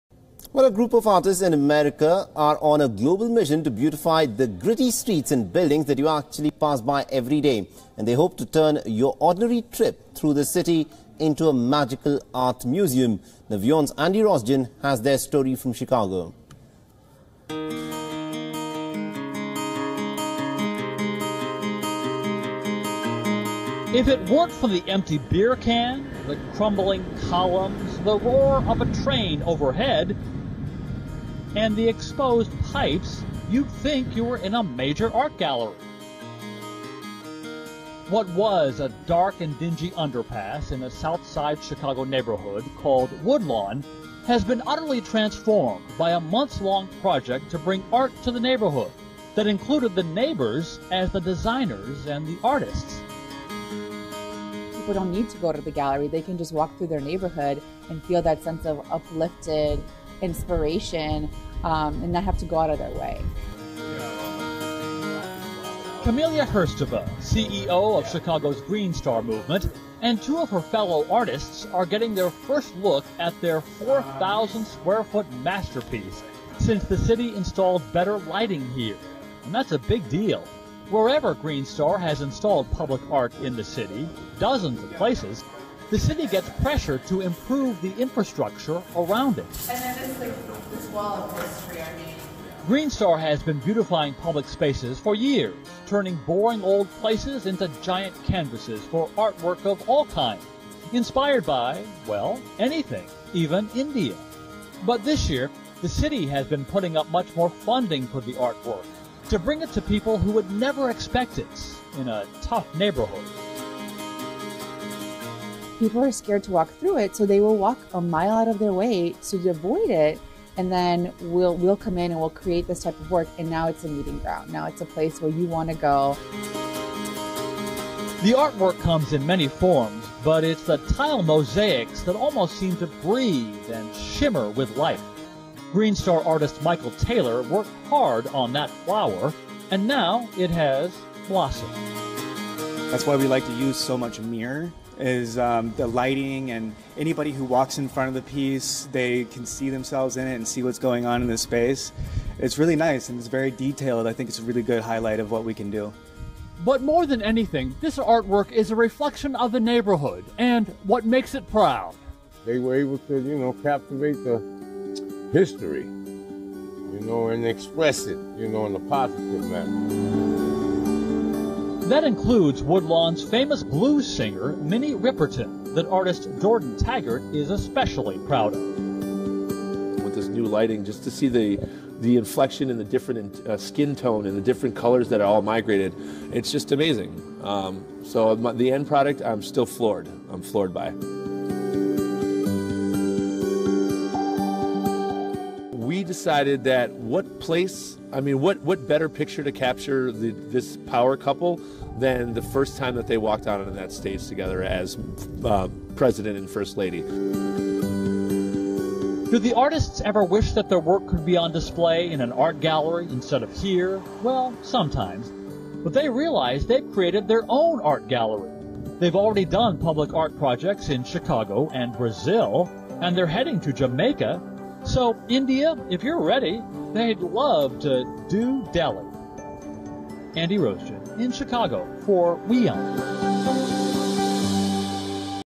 Group turns Chicago streets into art museum
A group of artists called Greenstar Movement is on a mission to turn streets into a art museum. These artists are beautifying Chicago's streets and buildings.